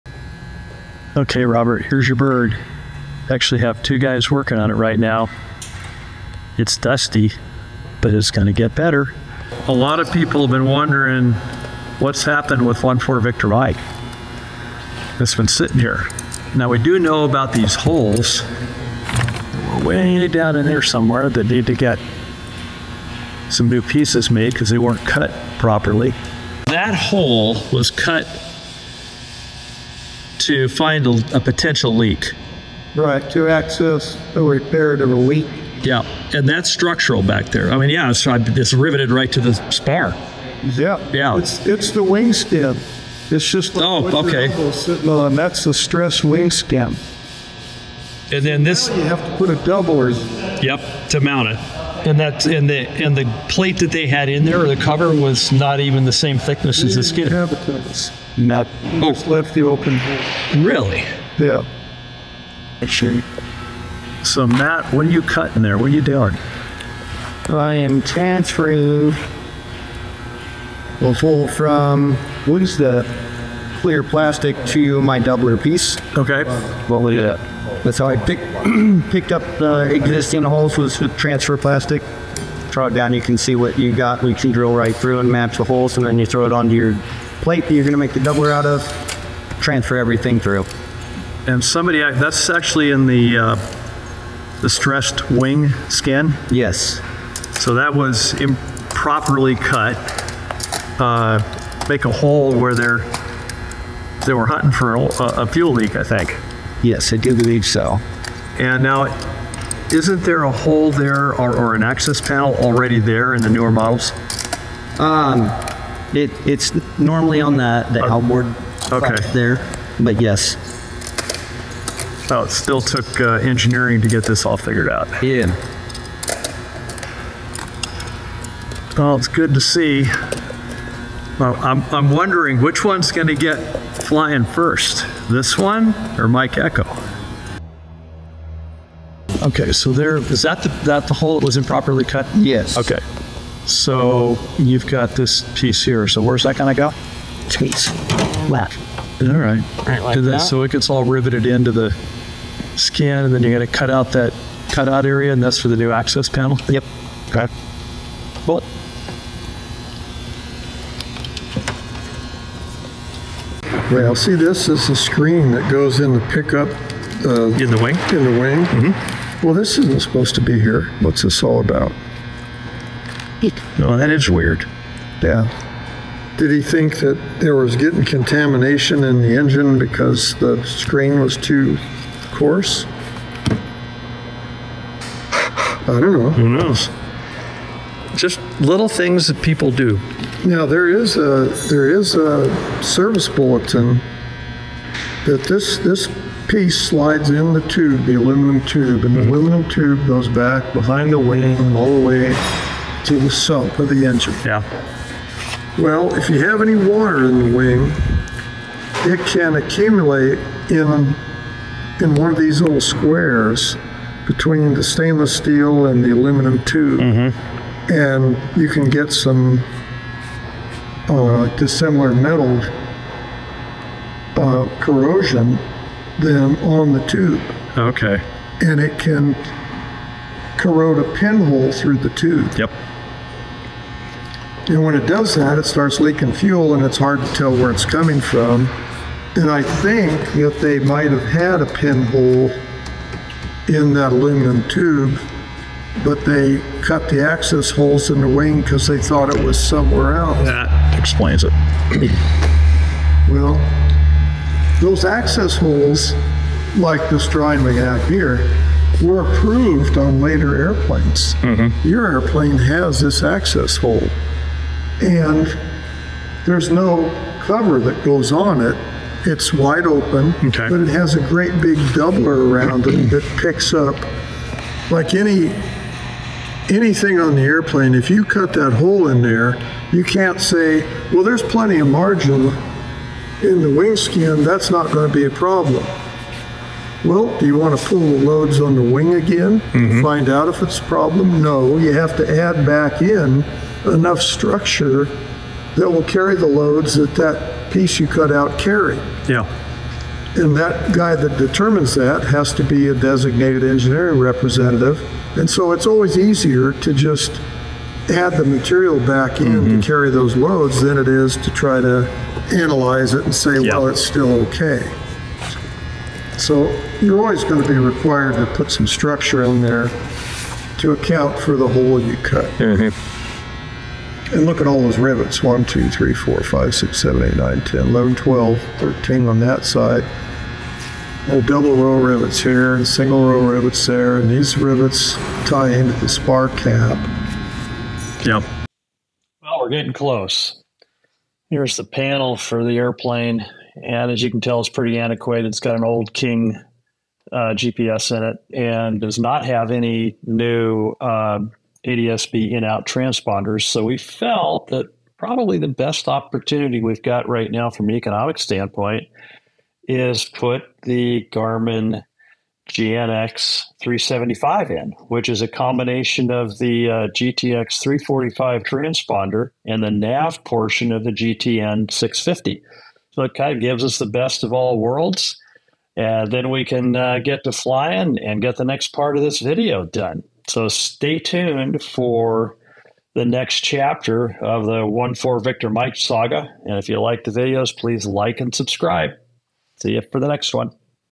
Reviving the LEGENDARY Aerostar Superstar 700 to New Heights!
Getting the Aerostar Superstar 700 Airborne Again! | Engineering, Avionics, and Performance

We're excited to share that our Aerostar Superstar 700 is nearly ready to take to the skies! 🛫 When restoring a plane, it's crucial not to cut holes in the skin without proper engineering analysis – and we're making sure to do it right. In just a few weeks, this amazing aircraft should be airworthy again.

Currently, the avionics are almost original, so we're upgrading to an ADS-B transponder. Our plan is to install the Garmin GNX 375, a fantastic all-in-one box that combines the GTX-345 and the GTN 650 without com. Plus, it connects directly to your iPad – no need for Flight Stream!

Why the Aerostar Superstar 700?
The Aerostar is the fastest light piston twin, holding two world speed records:
✈️ 1000k closed course at 259 kts
✈️ 2000k closed course at 277 kts

With excellent factory support and a strong owners' community, it's an incredible aircraft to own and fly.

Performance Specs of the Aerostar 700:

True Airspeed (TAS) @ 65% Power: 235 kts, FL 200, 44 gph
Max Seating: 6 (certified for 7)
Suitable for Basic Med Pilots
Time Between Overhauls (TBO): 1800 hours
Max Cabin Pressure Differential: 5.5 PSI
Cabin Altitude: 8000 ft at FL250, 10,700 ft at FL300
Max Gross Take-Off Weight: 6850 lbs
Typical Useful Load: 2300 lbs
Max Fuel with Auxiliary Tank: 209.5 gallons
Resources for Aerostar Owners & Enthusiasts:
🌐 Aerostar Aircraft Corporation
📣 Aerostar Owners Association / @aerostarownersassociation